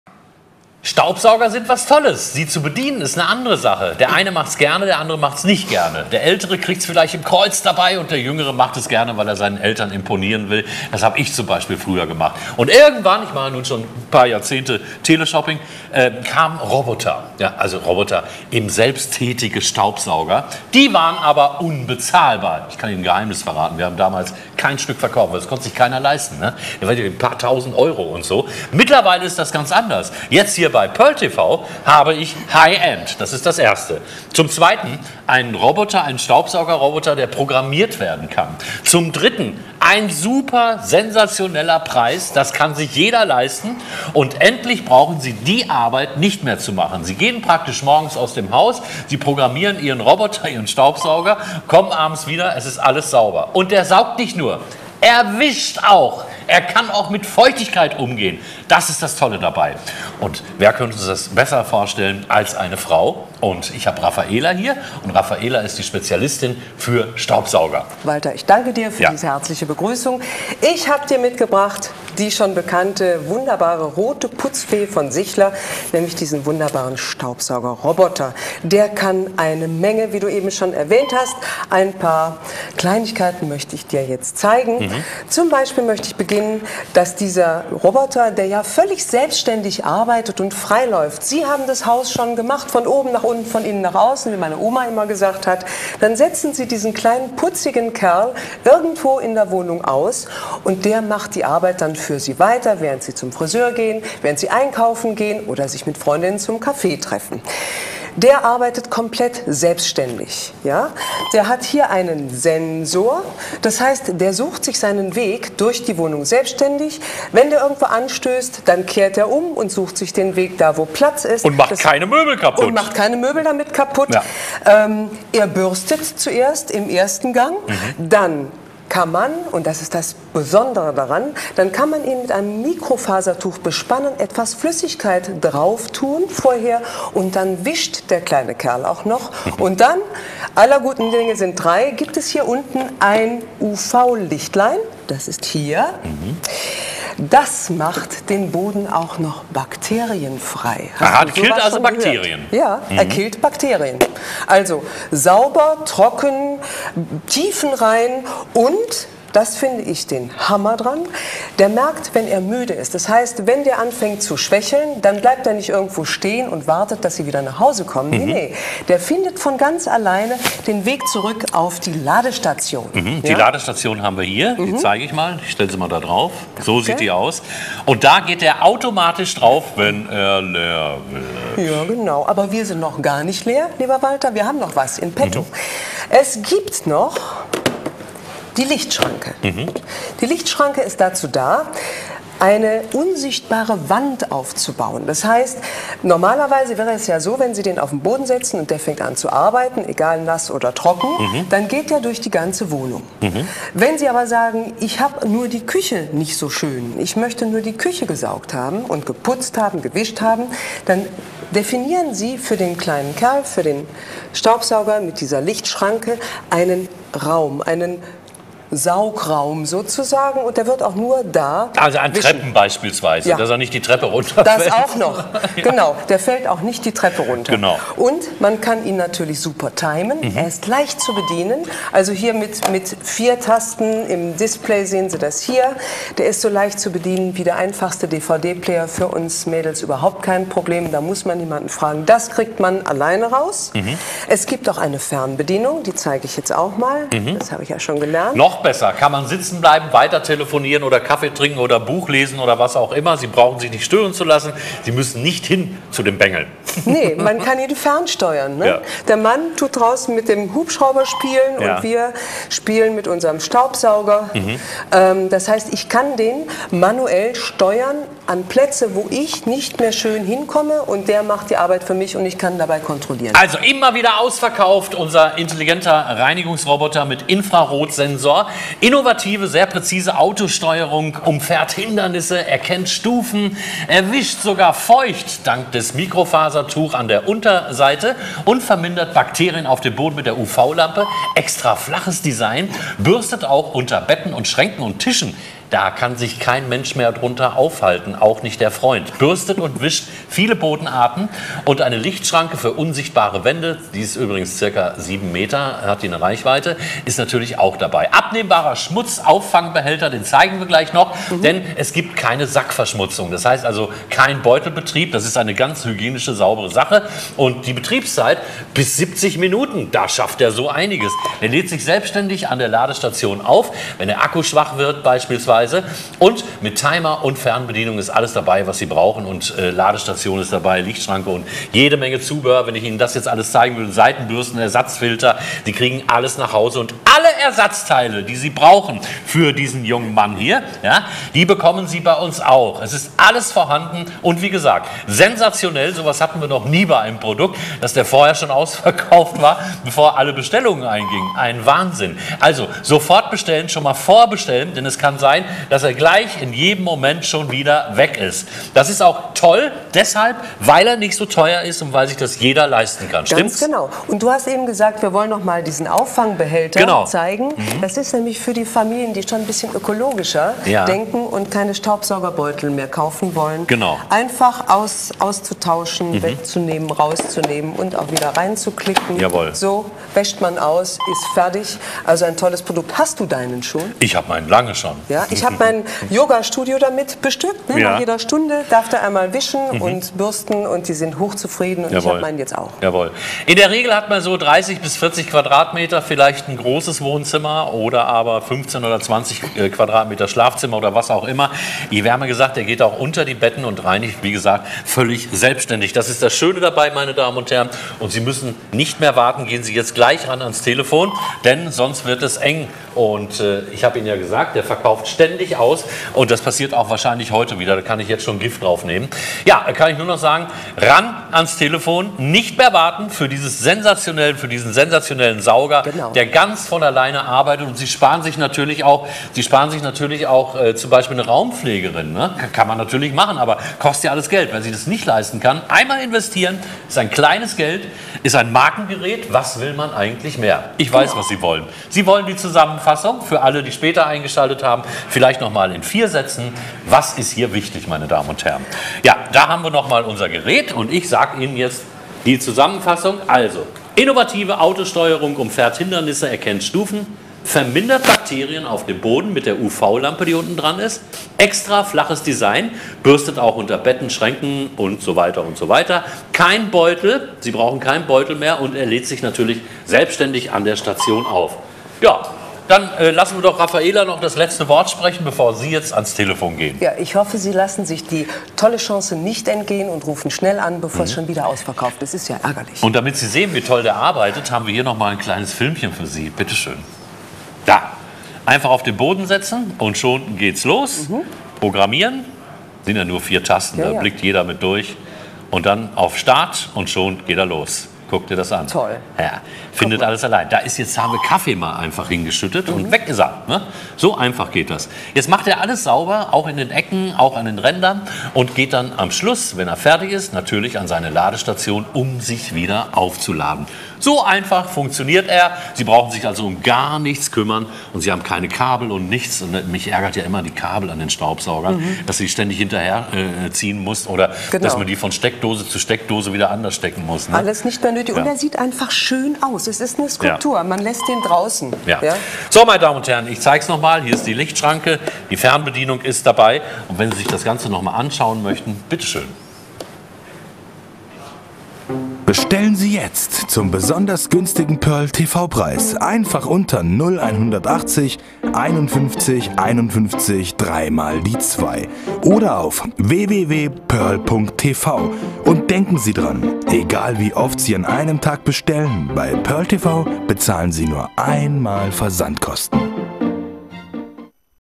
Sichler Reinigungs- und Staubsauger-Roboter PCR-3350.UV mit Wisch-Mopp
Ob nass aufputzen oder trocken durchwischen: Ihr neuer Reinigungs-Roboter sorgt vollautomatisch für Sauberkeit in der ganzen Wohnung. Lassen Sie sich die lästige Hausarbeit einfach abnehmen, egal ob auf Stein, Teppich, Parkett oder Fliesenboden.  Schnell feucht aufgewischt: Einfach das Mikrofasertuch anfeuchten und schon putzt der kleine Mopp automatisch Ihren Boden.  Nicht nur sauber, sondern tiefenrein: Die UV-Lampe an der Unterseite des Reinigungs-Roboters vermindert sogar Bakterien auf dem Boden!  Einfach schön, wenn man spielend so viel Zeit gewinnt: Ihre kleine intelligente Putzfee bürstet und wischt automatisch. Auch unter Möbeln. Dabei umfährt der Roboter Hindernisse und erkennt sogar Treppenstufen.   Zusätzlich verfügt dieses Modell über einen praktischen Timer. So legt Ihre neue Putzhilfe genau dann mit der Arbeit los, wenn Sie es wünschen.  Mit der kleinen Fernbedienung steuern Sie den Mini-Saubermann auf Wunsch mühelos vom Sofa aus in die staubigsten Ecken.  Nur schnell die Küche putzen? Kein Problem: Eine Infrarot-Lichtschranke baut dem Putzroboter eine unsichtbare Wand in den Weg. So können Sie den zu reinigenden Bereich nach Ihren Wünschen definieren.  * Intelligenter Reinigungs-Roboter mit Infrarotsensor * Innovative, sehr präzise Auto-Steuerung: Umfährt Hindernisse, erkennt Stufen * Wischt sogar feucht auf: Mikrofasertuch an der Unterseite * Vermindert Bakterien auf dem Boden: Mit UV-Lampe * Extra flaches Design: Bürstet auch unter Betten, Schränken und Tischen  * Effektives Reinigungssystem: Bürstet und wischt viele Bodenarten (ideal für Parkett, Laminat, kurzflorige Teppiche, Linoleum, Stein, Fliesen, Glas u.v.m.)  * IR-Lichtschranke für "unsichtbare Wände", ca. 7 Meter Reichweite * Insgesamt 26 hochsensible Sensoren, davon 24 zur Erkennung der Umgebung (Wände, Hindernisse, Abgründe)  & 2 zur Schmutzerkennung * Beutel-frei: Abnehmbarer Schmutz-Auffangbehälter * Lange Betriebszeit von ca. 50-70 Minuten * Ladestation mit LCD-Display * Lädt sich selbstständig an der Ladestation auf, wenn der Akku schwach wird * Ladezeit zwischen den Saugvorgängen: 4 Stunden * Kompakte Maße: 350 x 350 x 100 mm * Mit Timer und Fernbedienung * Benötigt für die Fernbedienung 2 Batterien Micro (AAA) und für die Lichtschranke 2 Batterien Mono Typ D (LR20) * Inklusive Fernbedienung, Ladestation, Lichtschranke, 2 Seitenbürsten, Wischmopp-Halter, 2 Wischmopp-Mikrofasertücher, Ersatz-Filter, 6 Schaumstoff-Streifen, Reinigungsbürste, Netzteil und deutscher Anleitung